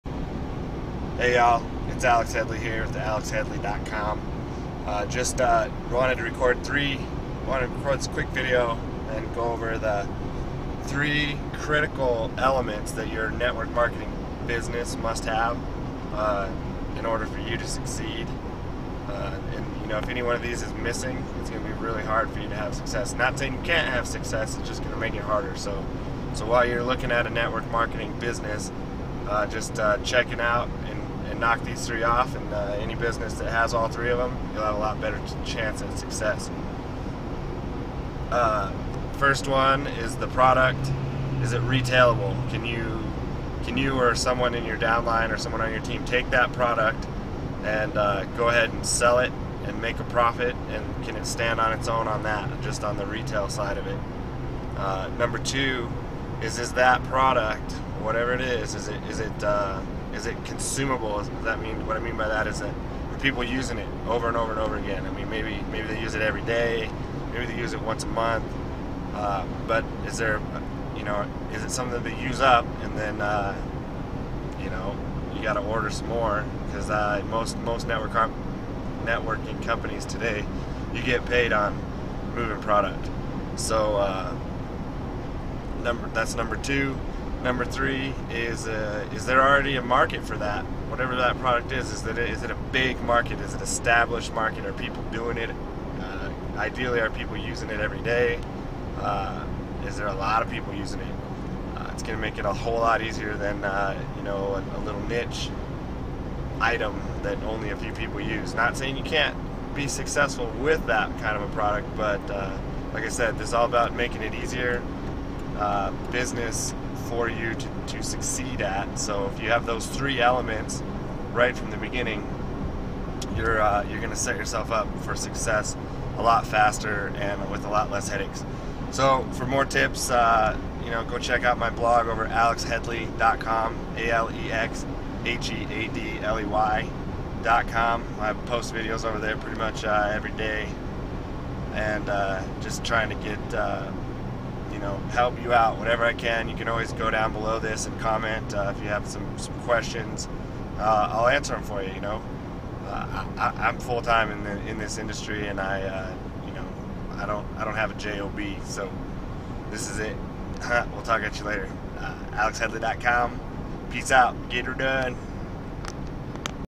3 Key Ingredients To Your Success in Network Marketing
Alex Headley talks about the 3 key ingredients that you need to have in your business to easily and quickly succeed in your network marketing business.